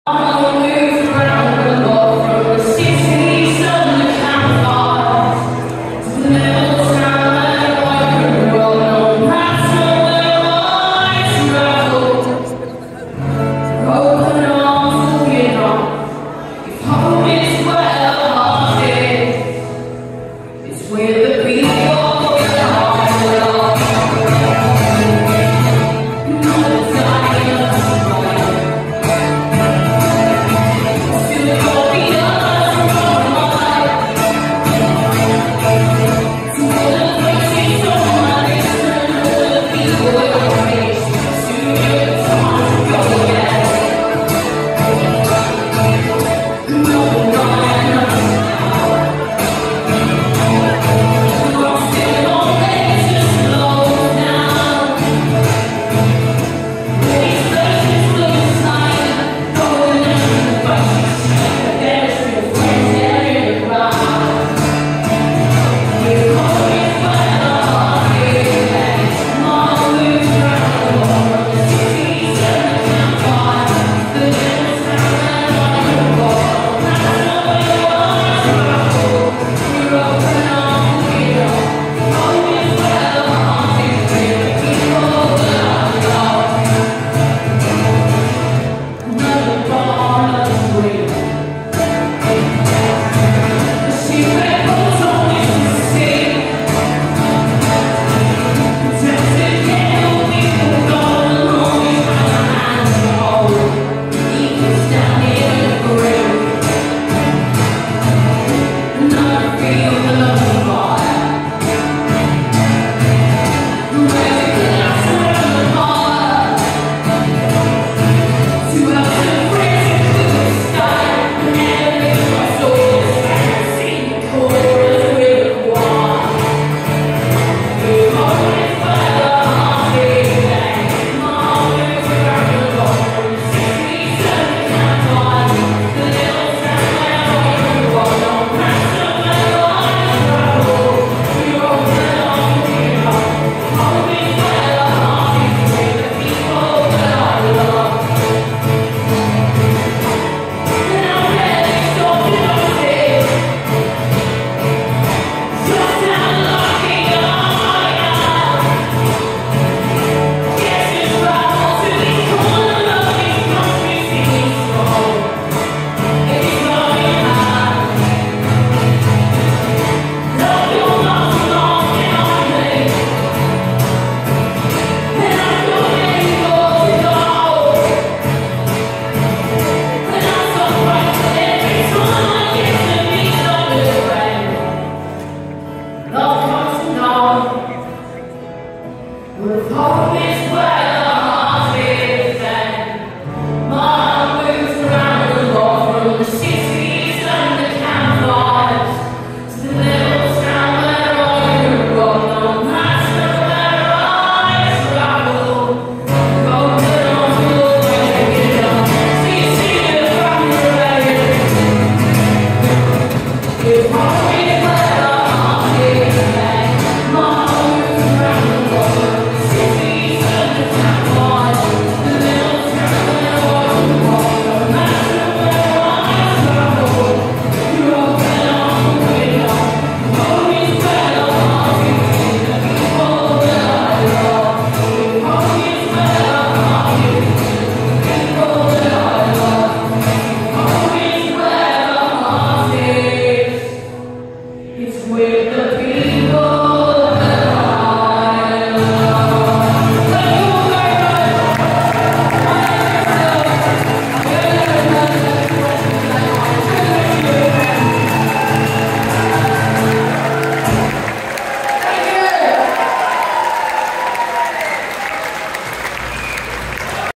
Jess Silk 6 live at Rebellion Punk Festival in Blackpool, crowd group hug to end set 2023
What do you think? Do you know the names of the tunes I don't? Were you there too?